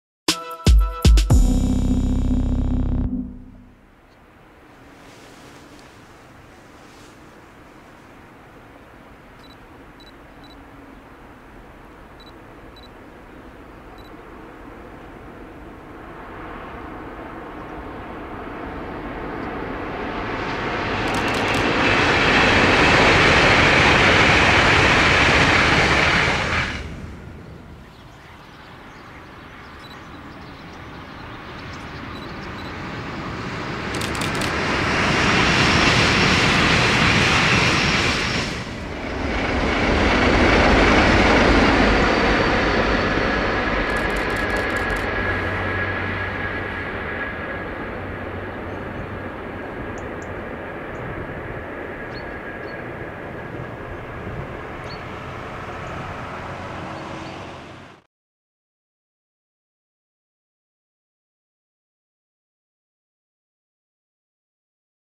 ETR 675 di NTV in transito sulle linee AV intorno a Roma
ETR 675 di NTV in transito sulle linee AV Firenze - Roma - Napoli. Video di Lorenzo Pallotta